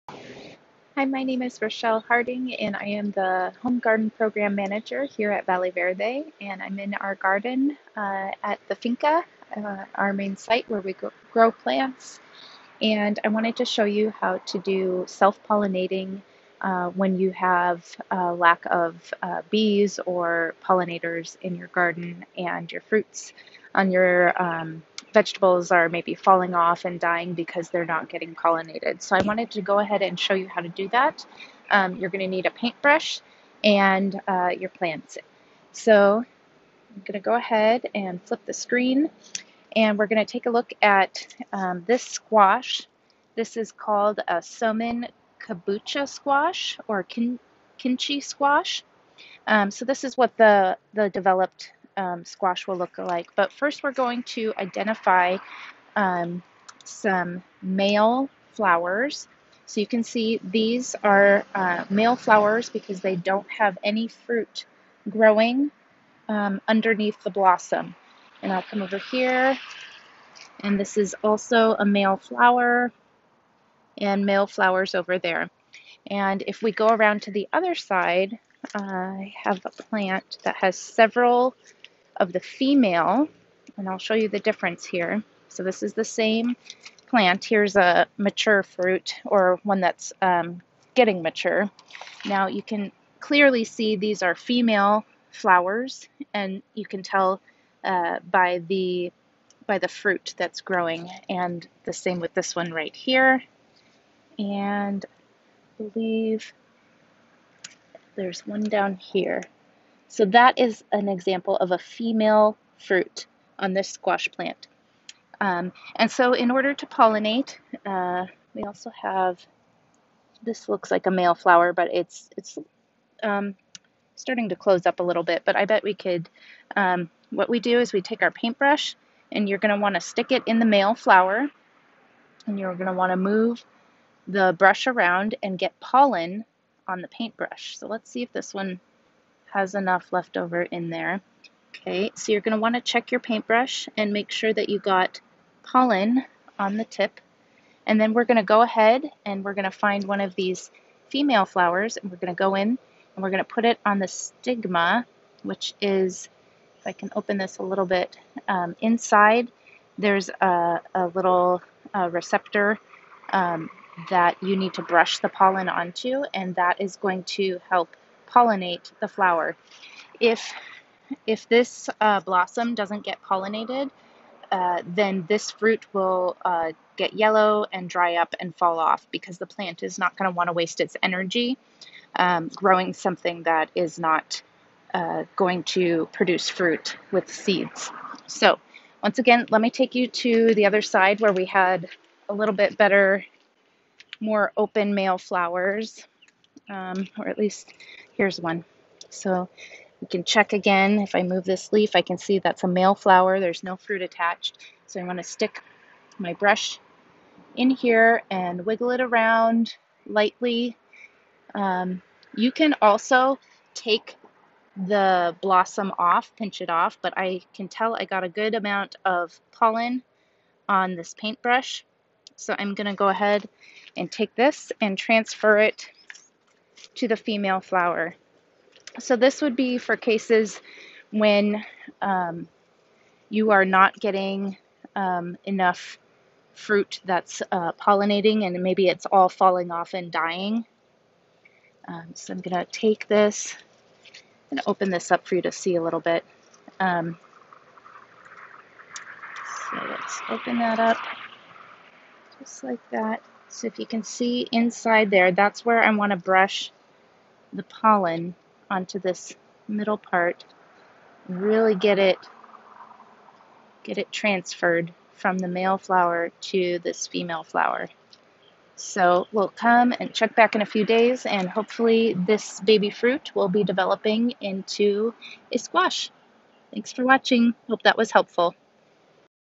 Valley Verde Garden Tips: Hand Pollination
In this video you will learn how to identify male and female squash flowers as well as how to hand pollinate. NOTE: The squash in this video was mislabeled! It is a spaghetti squash, not a kabocha black forest squash!! The kabocha black forest squash is round and dark green.